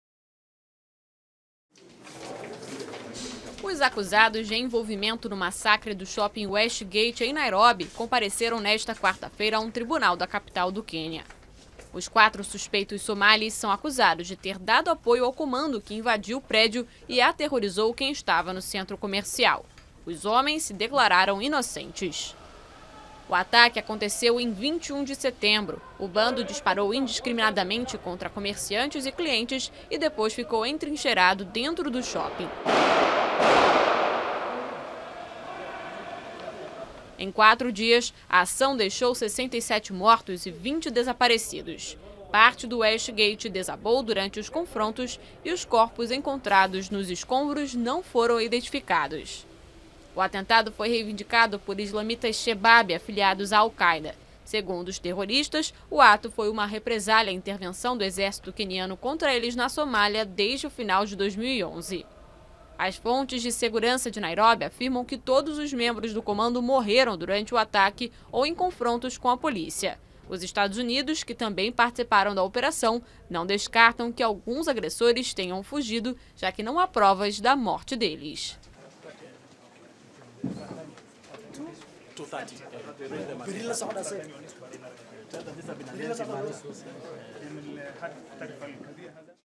Começa o julgamento do massacre no shopping Westgate
Começou nesta quarta-feira o julgamento de quatro homens acusados de ter dado apoio ao comando que conduziu a matança no shopping Westgate em Nairóbi no final de setembro.